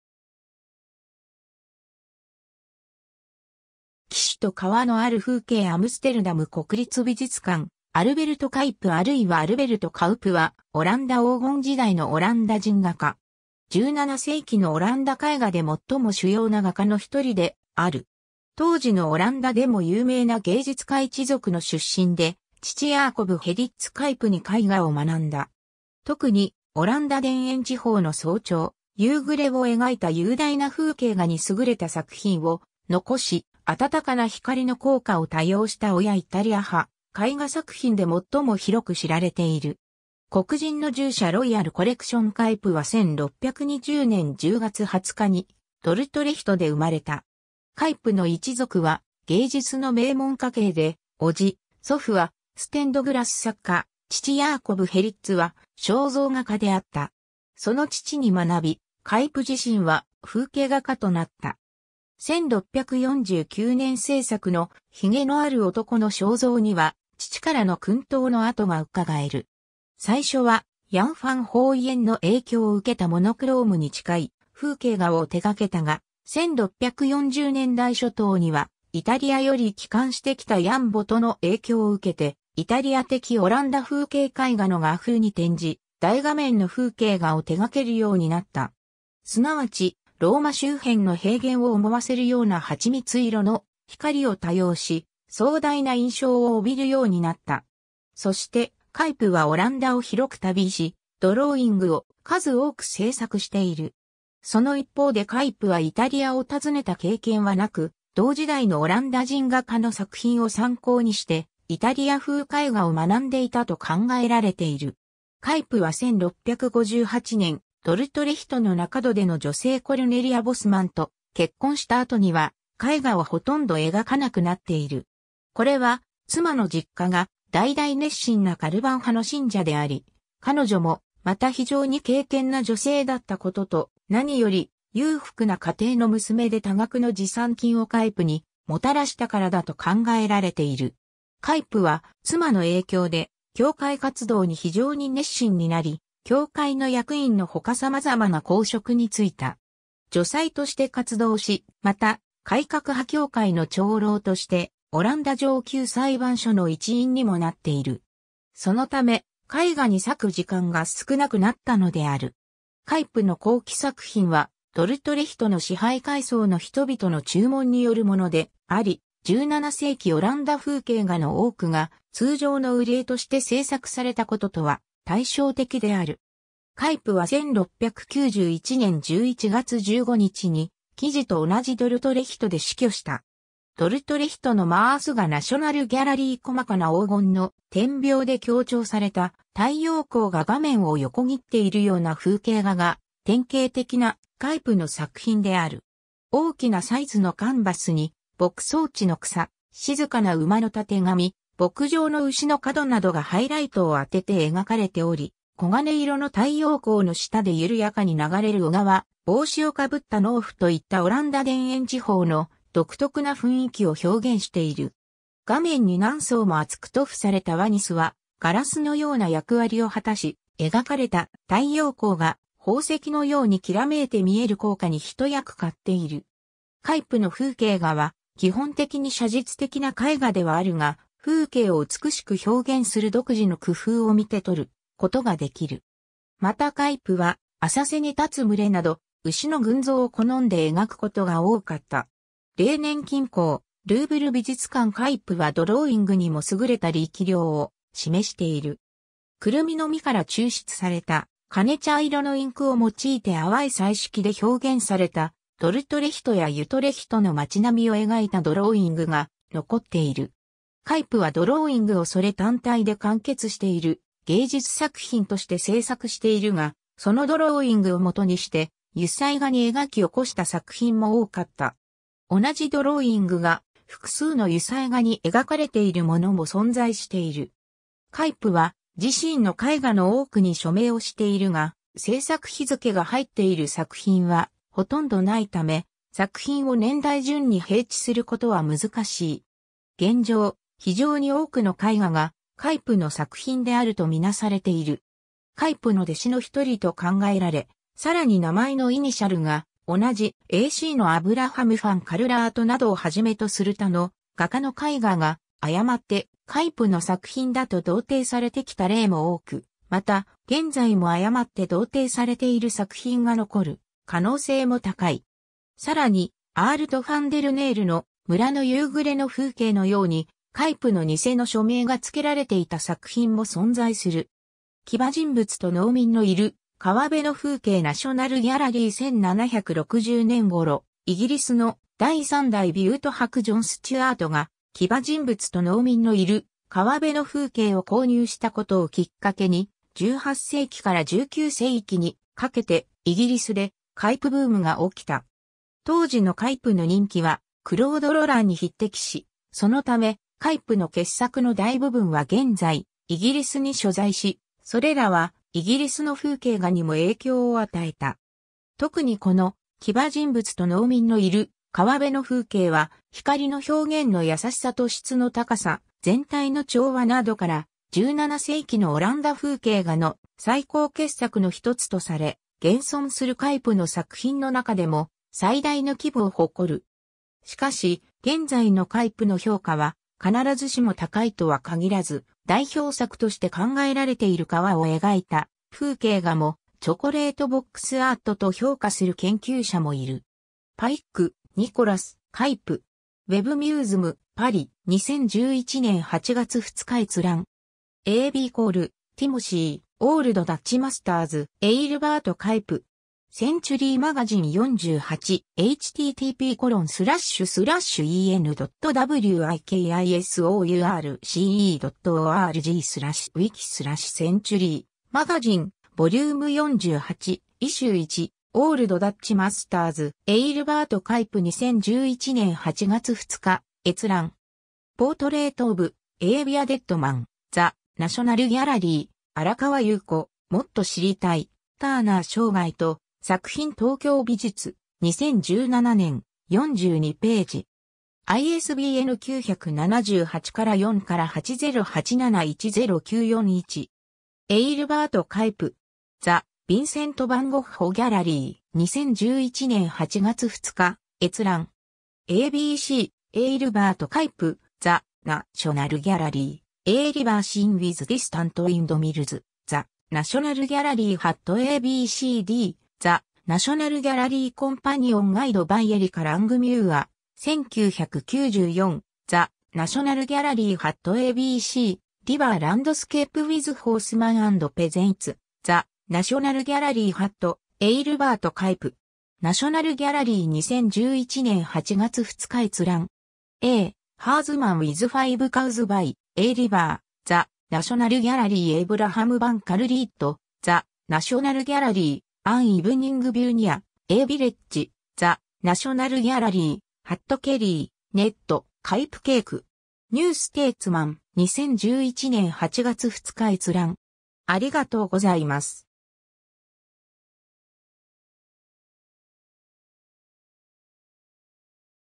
アルベルト・カイプ
『騎手と川のある風景』（1655年頃） アムステルダム国立美術館 アルベルト・カイプあるいはアルベルト・カウプ（オランダ語: Aelbert Cuyp, 1620年10月20日 - 1691年11月15日）は、オランダ黄金時代のオランダ人画家。
17世紀のオランダ絵画でもっとも主要な画家の一人である。
当時のオランダでも有名な芸術家一族の出身で、父ヤーコブ・ヘリッツ・カイプ (en:Jacob Gerritsz. Cuyp) に絵画を学んだ。
特にオランダ田園地方の早朝、夕暮れを描いた雄大な風景画に優れた作品を残し、温かな光の効果を多用した親イタリア派絵画作品で最も広く知られている。
 『黒人の従者』（1652年頃） ロイヤル・コレクション カイプは1620年10月20日にドルトレヒトで生まれた。
カイプの一族は芸術の名門家系で、叔父、祖父はステンドグラス作家、父ヤーコブ・ヘリッツは肖像画家であった。
その父に学び、カイプ自身は風景画家となった。
1649年制作の『髭のある男の肖像』（"Portrait of a Bearded Man"）には、父からの薫陶の跡がうかがえる。
 最初はヤン・ファン・ホーイェンの影響を受けたモノクロームに近い風景画を手がけたが、1640年代初頭には、イタリアより帰還してきたヤン・ボト(en:Jan Dirksz Both) の影響を受けて、「イタリア的オランダ風景絵画」（Dutch-Italianate、親イタリア派、イタリアネート派）の画風に転じ、大画面の風景画を手掛けるようになった。
すなわち、ローマ周辺の平原を思わせるような蜂蜜色の光を多用し、壮大な印象を帯びるようになった。
そして、カイプはオランダを広く旅し、ドローイングを数多く制作している。
その一方でカイプはイタリアを訪ねた経験はなく、同時代のオランダ人画家の作品を参考にして、イタリア風絵画を学んでいたと考えられている。
 カイプは1658年、ドルトレヒトの名門出の女性コルネリア・ボスマンと結婚した後には、絵画をほとんど描かなくなっている。
これは妻の実家が代々熱心なカルヴァン派（改革派）の信者であり、彼女もまた非常に敬虔な女性だったことと、何より裕福な家庭の娘で多額の持参金をカイプにもたらしたからだと考えられている。
カイプは妻の影響で教会活動に非常に熱心になり、教会の役員のほか様々な公職に就いた。
助祭として活動し、また、改革派教会の長老としてオランダ上級裁判所の一員にもなっている。
そのため、絵画に割く時間が少なくなったのである。
 カイプの後期作品はドルトレヒトの支配階層の人々の注文によるものであり、17世紀オランダ風景画の多くが通常の売り絵として制作されたこととは対照的である。
カイプは1691年11月15日に生地と同じドルトレヒトで死去した。
 『ドルトレヒトのマース河』（1660年頃） ナショナル・ギャラリー (ワシントン) 細かな黄金の点描で強調された太陽光が画面を横切っているような風景画が、典型的なカイプの作品である。
大きなサイズのカンバスに、牧草地の草、静かな馬のタテガミ、牧場の牛の角などがハイライトを当てて描かれており、黄金色の太陽光の下で緩やかに流れる小川、帽子を被った農夫といったオランダ田園地方の独特な雰囲気を表現している。
画面に何層も厚く塗布されたワニスはガラスのような役割を果たし、描かれた太陽光が宝石のようにきらめいて見える効果に一役買っている。
カイプの風景画は基本的に写実的な絵画ではあるが、風景を美しく表現する独自の工夫を見てとることが出来る。
またカイプは、浅瀬に立つ群れなど、牛の群像を好んで描くことが多かった。
 『レーネン近郊』（1650年 - 1655年頃） ルーブル美術館 カイプはドローイングにも優れた力量を示している。
クルミの実から抽出された金茶色のインクを用いて淡い彩色で表現されたドルトレヒトやユトレヒトの町並みを描いたドローイングが残っている。
カイプはドローイングをそれ単体で完結している芸術作品として制作しているが、そのドローイングをもとにして油彩画に描き起こした作品も多かった。
同じドローイングが複数の油彩画に描かれているものも存在している。
 カイプは自身の絵画の多くに署名をしているが制作日付が入っている作品はほとんどないため、作品を年代順に並置することは難しい。
現状、非常...